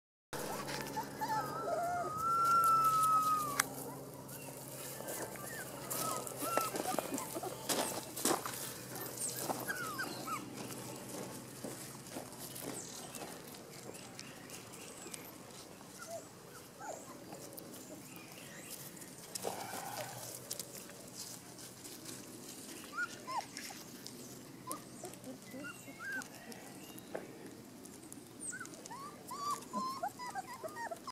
Indy Golden Retriever dad With three puppies
This short video shows Indy, the dad of 12 golden retriever puppies, watching over the brood--one of whom strayed slightly. Indy goes over to check on Big Boy...